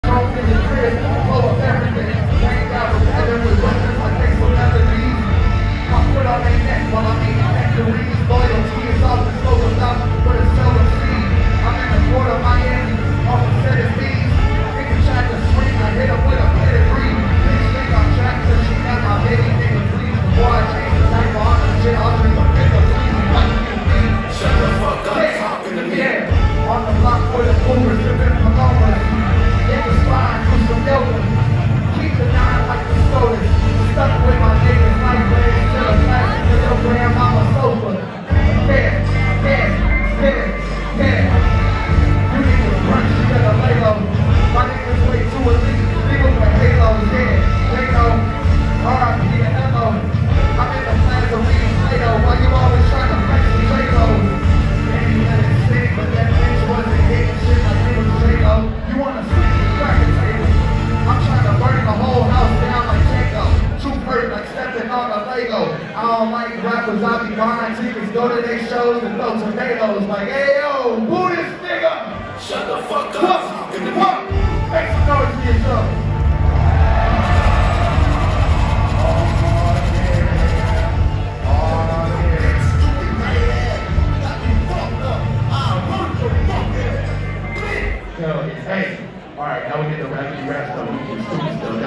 Zack Fox Live in Concert | Shut the F**k Up Talking To Me | June 2, 2022
Performing at Showbox SoDo in Seattle Washington